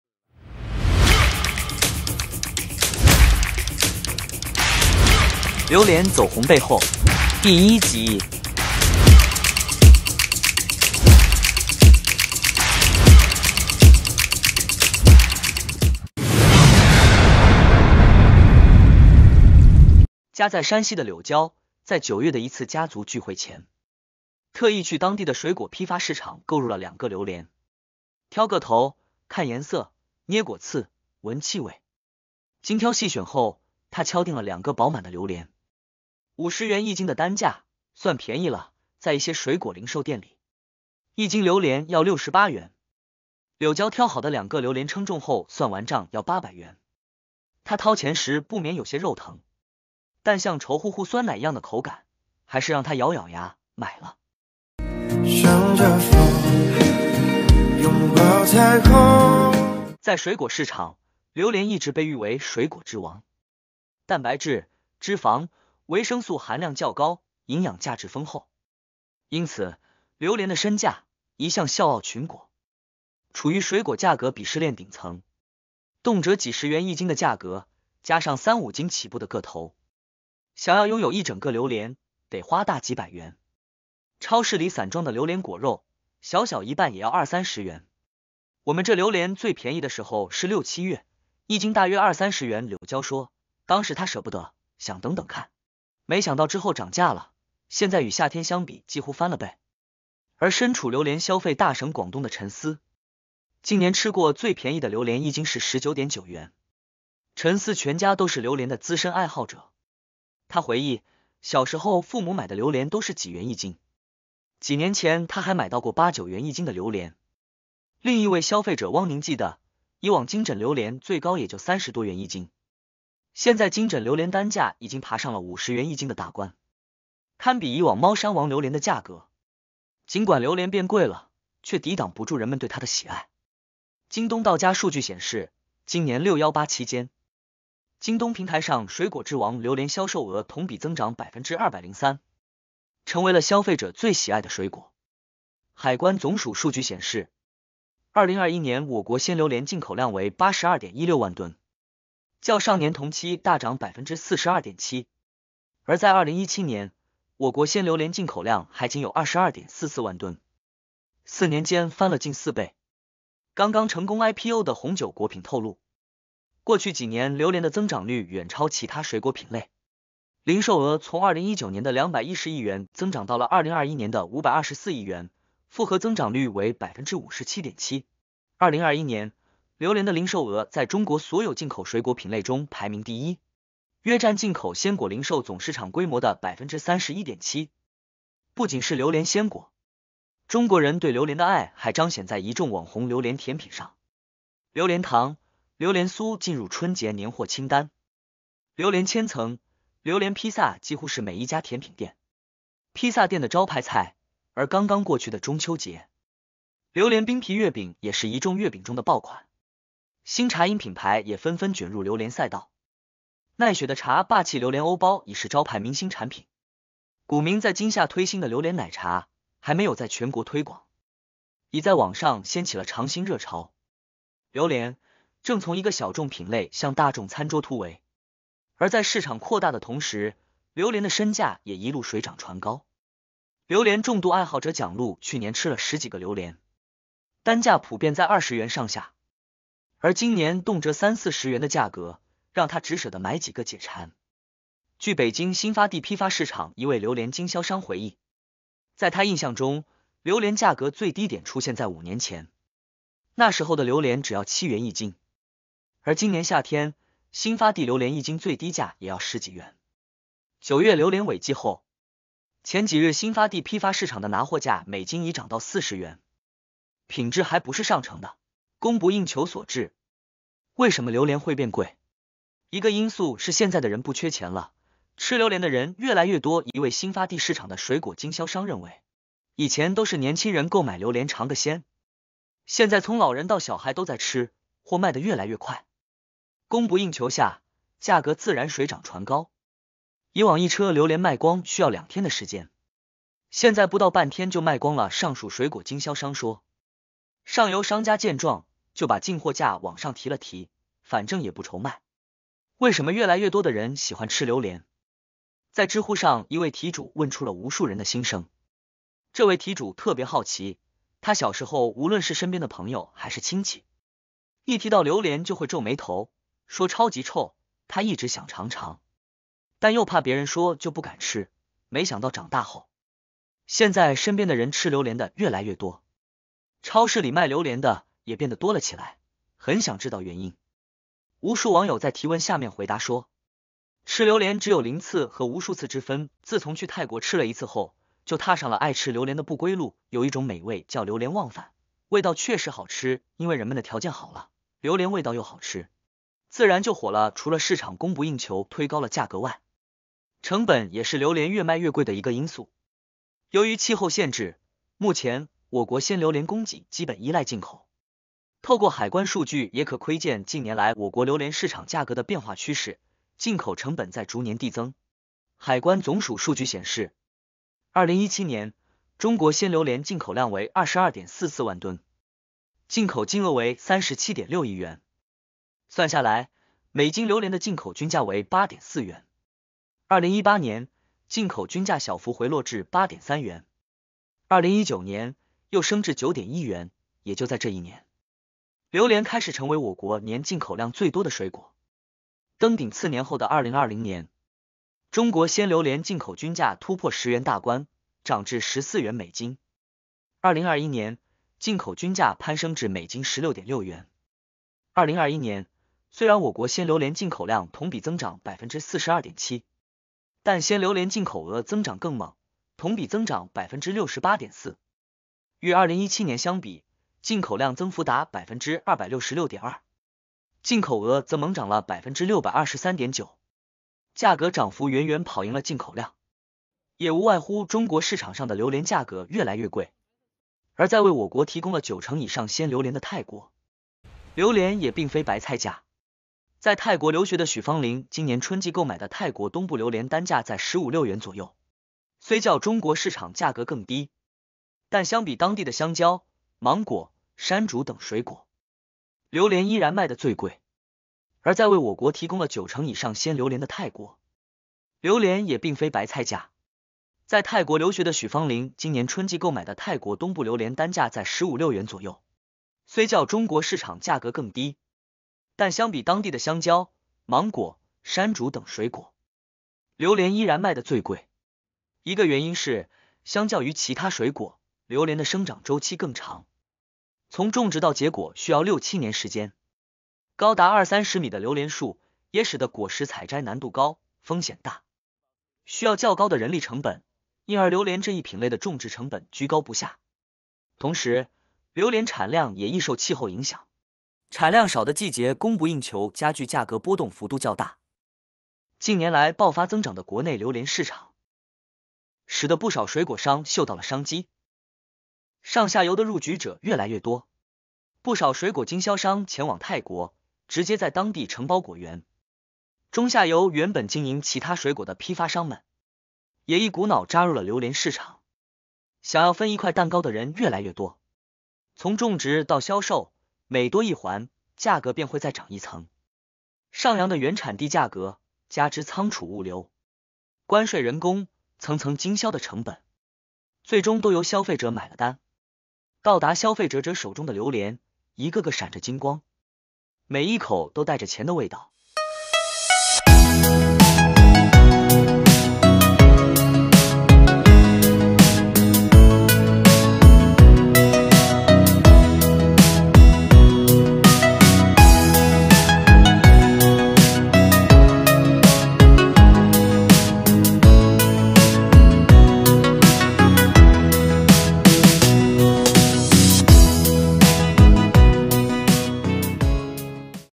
猫山王榴莲走红背后 ， 一年卖五百多亿，有人靠其发家致富
京东到家数据显示，今年618期间，京东平台上水果之王榴莲销售额同比增长203%，成为了消费者最喜爱的水果。
海关总署数据显示，2021年我国鲜榴莲进口量为82.16万吨，较上年同期大涨42.7%。而在2017年，我国鲜榴莲进口量还仅有22.44万吨，四年间翻了近四倍。